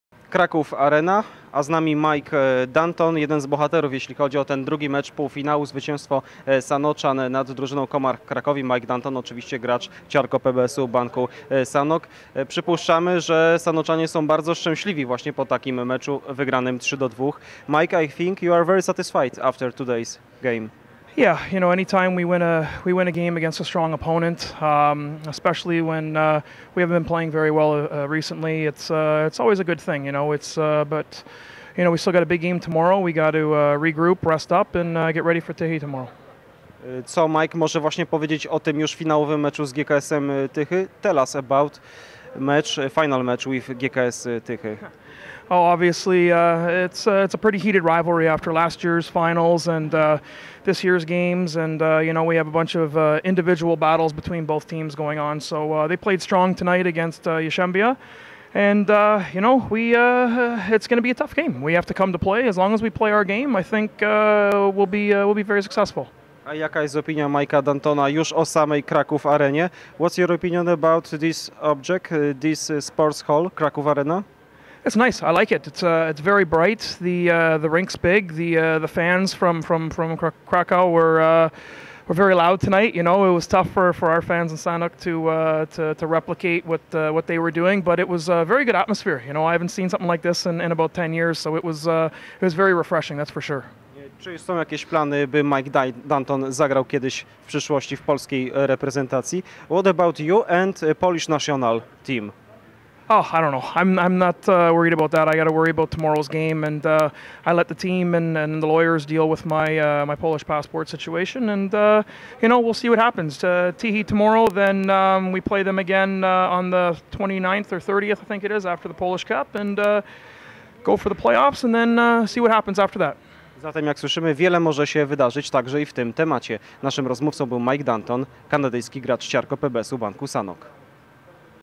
serwis rozmawia z Mikiem Dantonem
- Prawnicy zajmują się moim sprawami związanymi z polskim paszportem i zobaczymy co się wydarzy - tak Mike Danton odpowiada na pytanie o ewentualne występy w polskiej reprezentacji. Kanadyjski hokeista Ciarko PBS-u Banku Sanok podkreślił także po finałowym turnieju Pucharu Polski, że już dawno nie brał udziału w tak znakomitej imprezie.